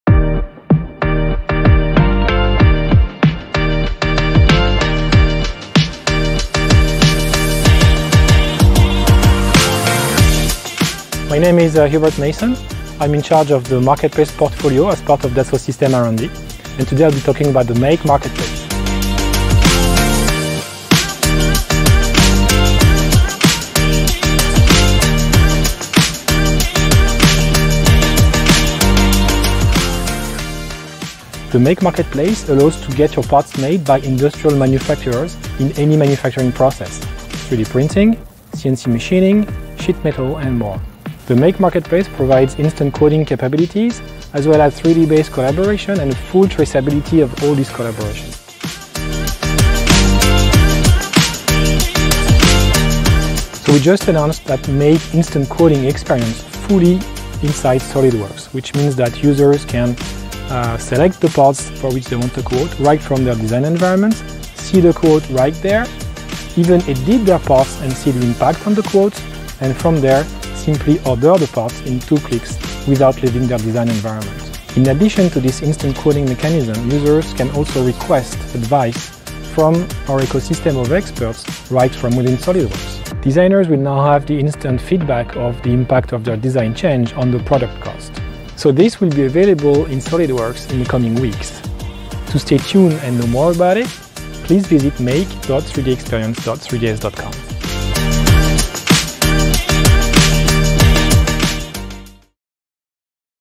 Exploring 3DEXPERIENCE MAKE from Dassault Systèmes
At 3DEXPERIENCE World 2020 we got to explore with Hubert Masson the 3DEXPERIENCE MAKE Marketplace from Dassault Systèmes! The MAKE platform is an on-demand manufacturing service that connects designers to 3D printing service providers across the world. The platform also provides instant quoting, which adjusts to your design changes.

Dassault Systèmes announced at the  3DEXPERIENCE World 2020 event that all SOLIDWORKS users will soon be able to access the instant quoting and design feedback mechanisms directly inside the software!